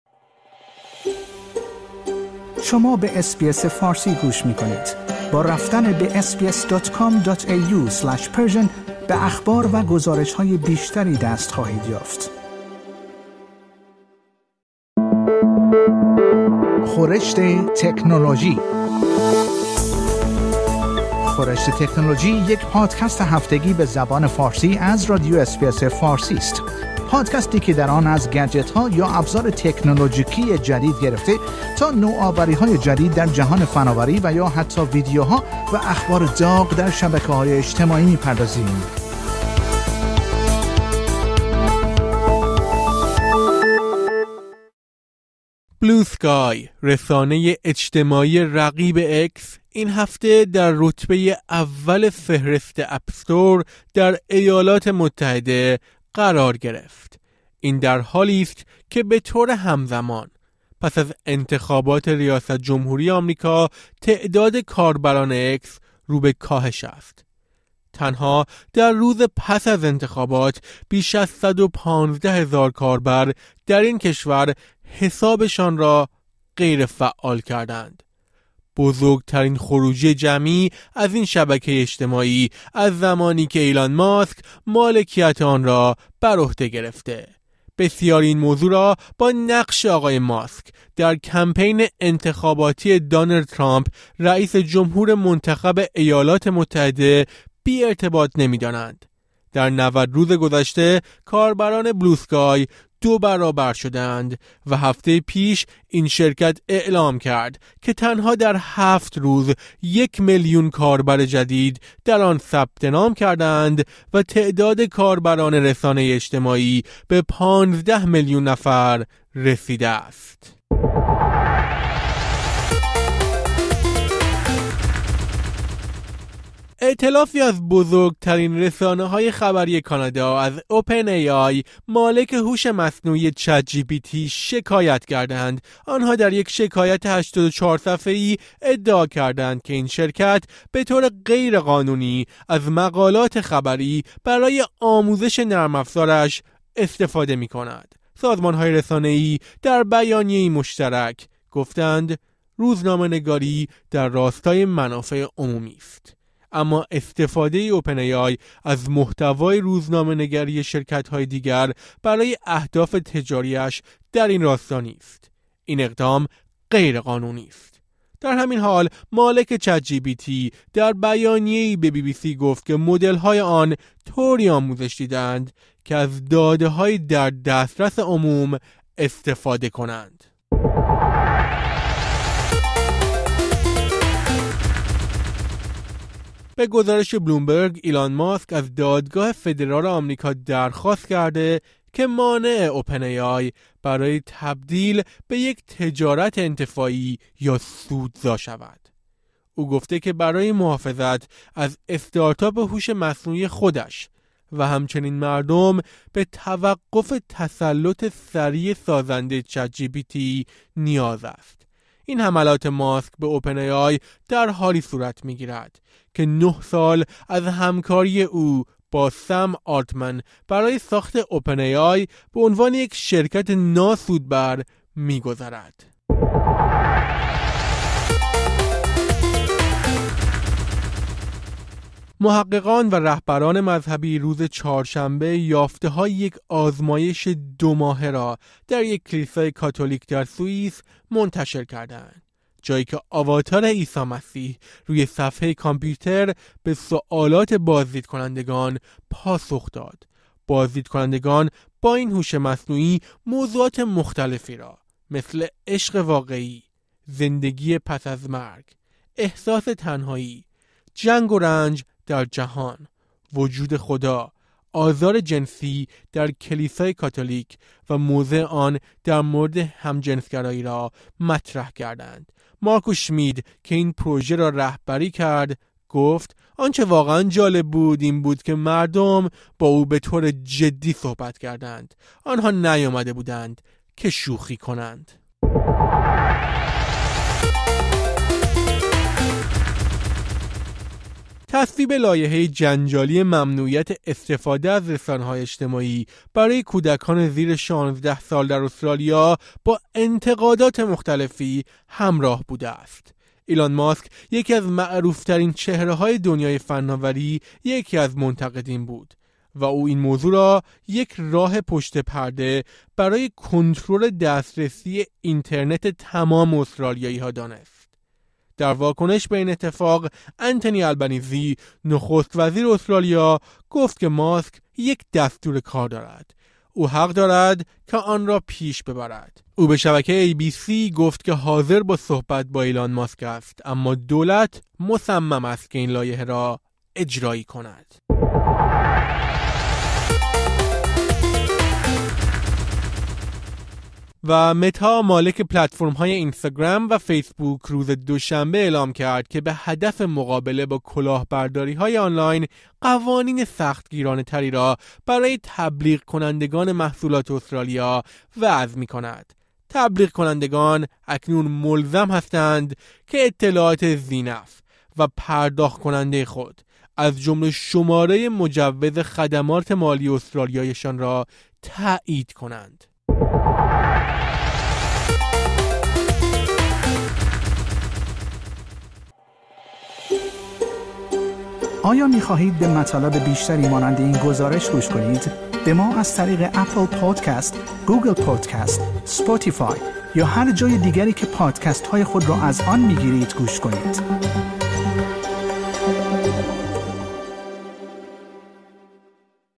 ادامه حملات قانونی ایلان ماسک به شرکت مالک هوش مصنوعی چت‌جی‌پی‌تی | SBS Persian - اس بی اس فارسی
از تحقیقی درباره هوش مصنوعی عیسی مسیح تا واکنش آلبانیزی به انتقاد ایلان ماسک از ممنوعیت رسانه‌های اجتماعی؛ این‌ها بخشی از مهمترین اخبار دنیای فناوری در یک هفته گذشته بودند.